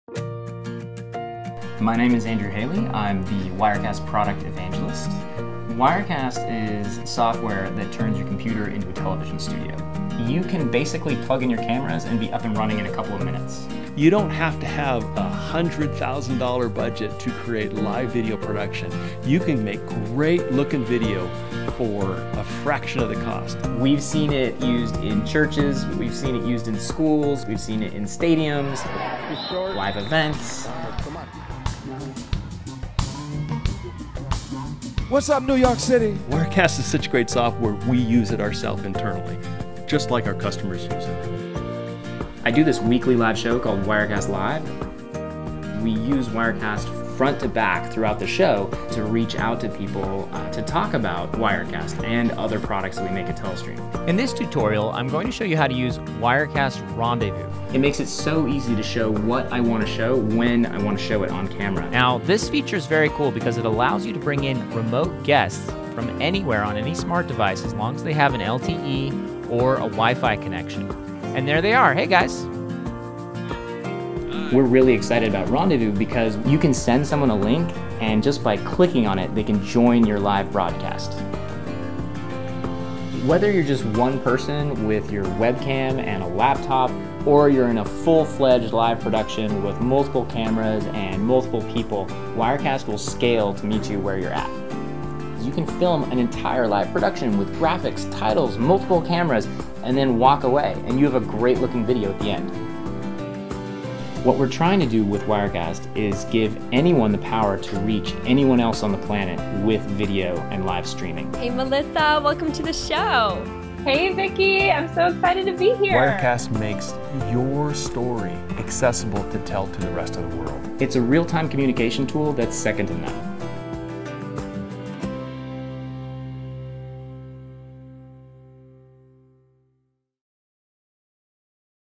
What is Wirecast?
What is Wirecast? Only a real-time communication tool that's second-to-none. Only one of the most popular, most award-winningest live streaming and production software solutions ever created. Only -- oh, just watch the video. It'll explain everything.

Comments are not moderated by Telestream. If you have a support question, go